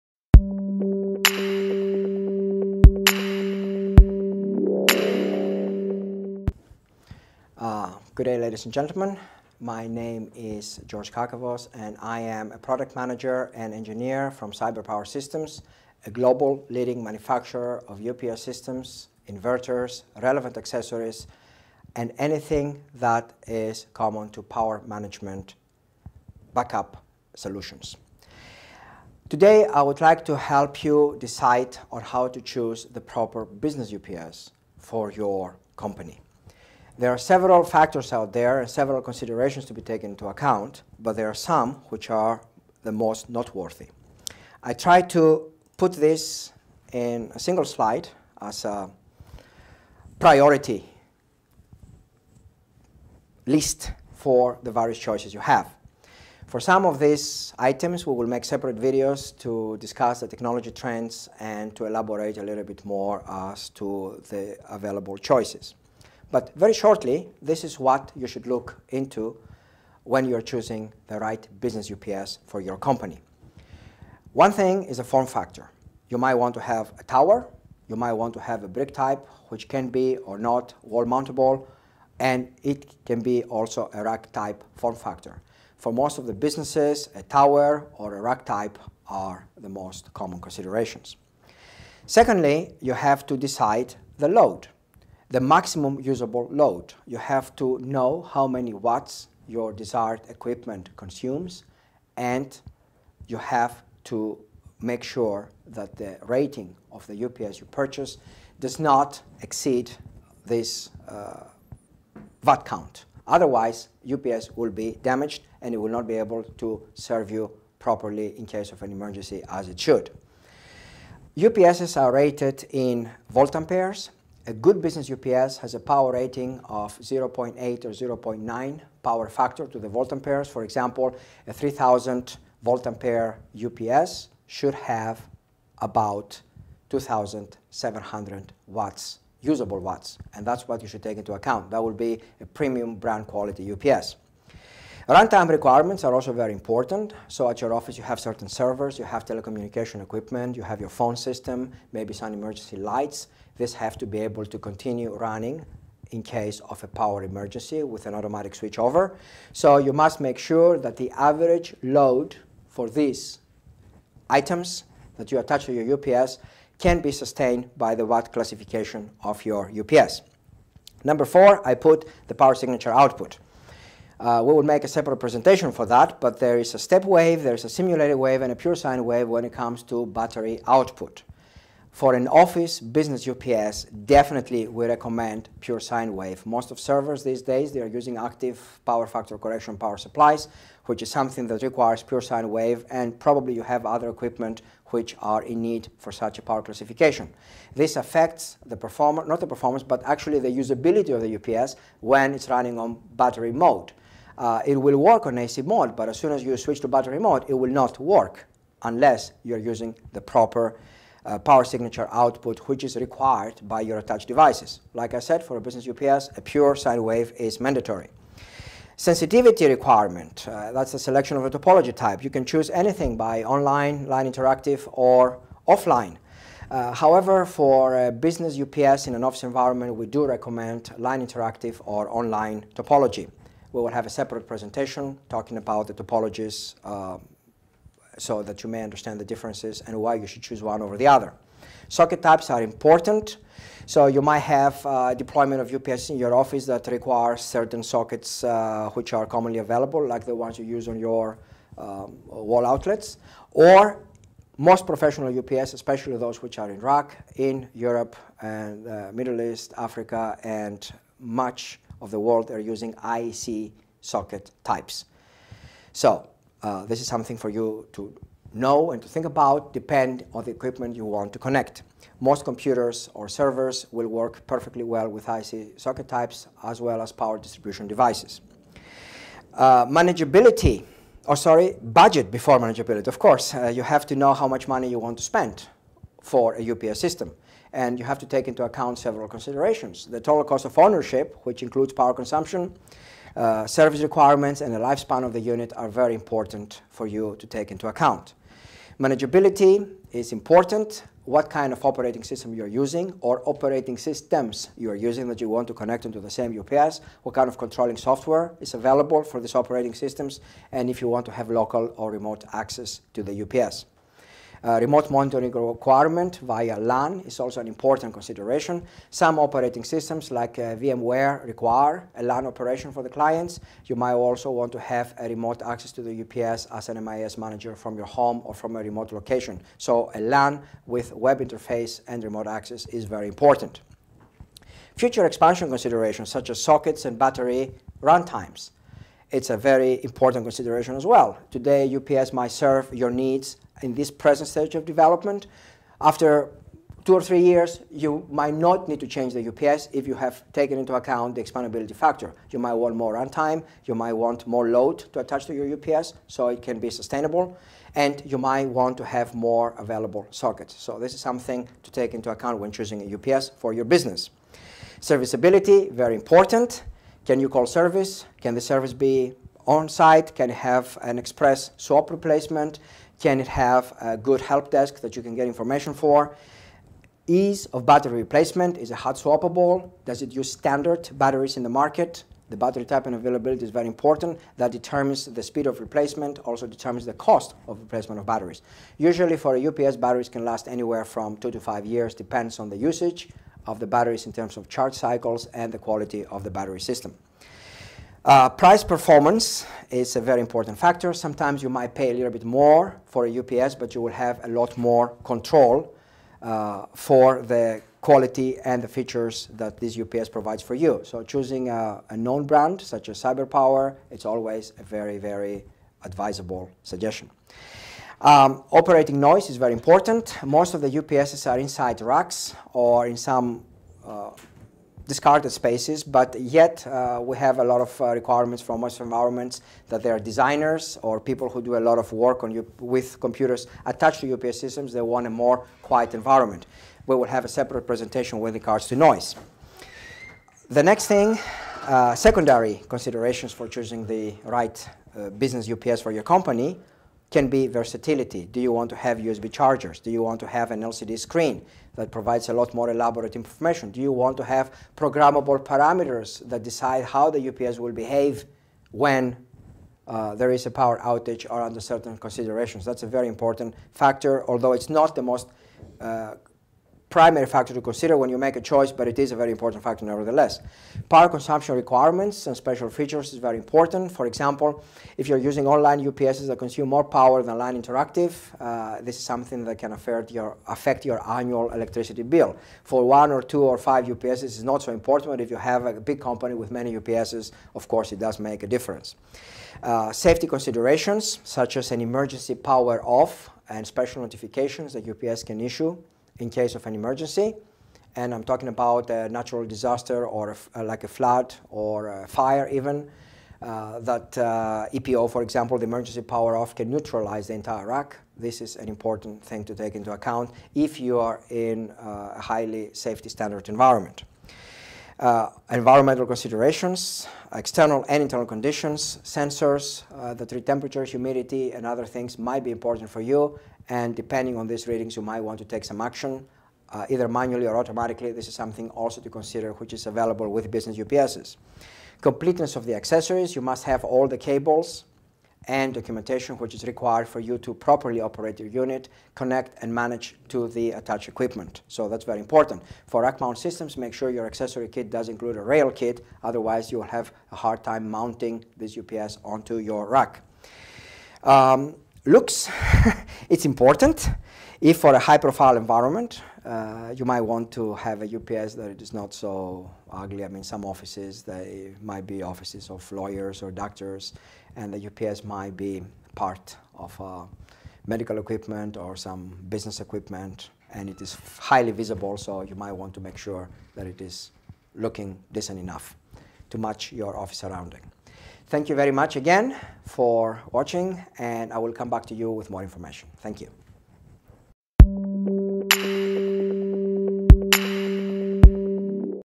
¿Cómo elegir el SAI adecuado para tu empresa?
Tercera parte de nuestra guía sobre SAIs. En este vídeo, George Carcavos de CyberPower Systems te explica que factores tienes que tener en cuenta a la hora de elegir un SAI para que compres el que mejor se adapte a tu empresa.
Y si quieres comprarlo un lugar de confianza y al mejor precio, pásate por nuestro catálogo: